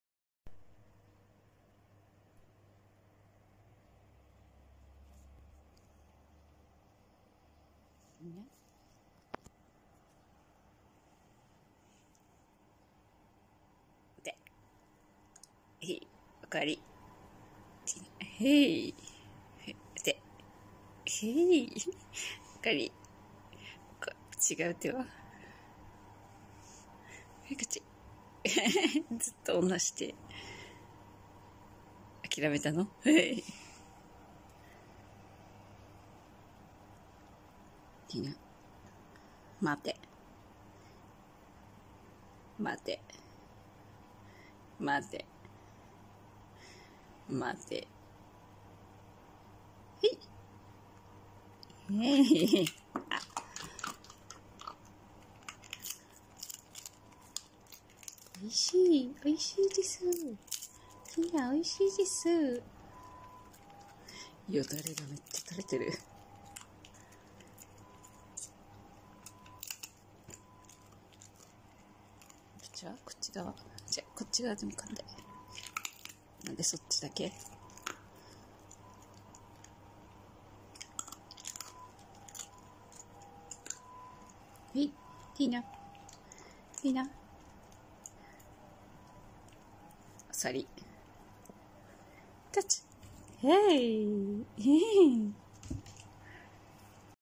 お手をすれば食べ物を貰えるとわかった犬の末路
ただひたすらにお手をします。横で何か食べてると勝手にお手をしてきます。